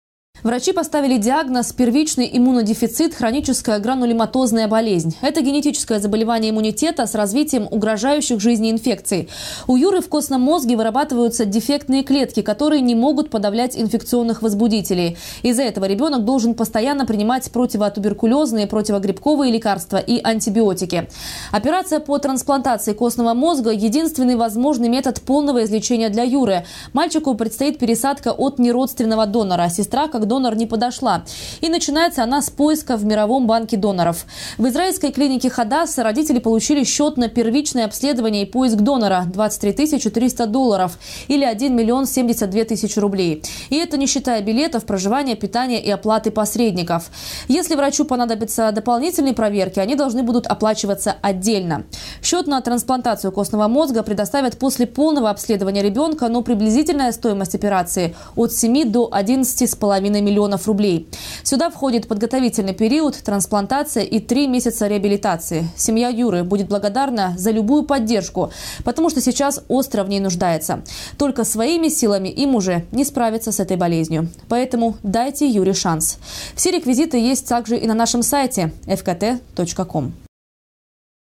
Маленькому сочинцу Юре Коваленко срочно нужны деньги на лечение Новости 24 Сочи Эфкате
Юре Коваленко требуется пересадка костного мозга.

Врачи поставили диагноз — первичный иммунодефицит, хроническая гранулематозная болезнь. Это генетическое заболевание иммунитета с развитием угрожающих жизни инфекций. У Юрия в костном мозге вырабатываются дефектные клетки, которые не могут подавлять инфекционных возбудителей. Из-за этого ребёнок должен постоянно принимать противотуберкулезные, противогрибковые лекарства и антибиотики. Операция по трансплантации костного мозга– единственный возможный метод полного излечения для Юры. Мальчику предстоит пересадка от неродственного донора (сестра как донор не подошла), начинается она с поиска в мировом банке доноров. В израильской клинике Хадасса родители получили счёт на первичное обследование и поиск донора — 23 тысячи 300 долларов (или 1 млн 72 тысячи рублей). Это не считая билетов, проживания, питания и оплаты посредников. Если врачу понадобятся дополнительные проверки, они должны будут оплачиваться отдельно. Счёт на трасплантацию костного мозга предоставят после полного обследования ребёнка, но приблизительная стоимость операции от семи до 11 с половиной миллионов рублей. Сюда входит подготовительный период, трансплантация и 3 месяца реабилитации. Семья Юры будет благодарна за любую поддержку, потому что сейчас остро в ней нуждается. Только своими силами им уже не справиться с этой болезнью. Поэтому дайте Юре шанс.

Сочи. Новости.
Телеканал Эфкате.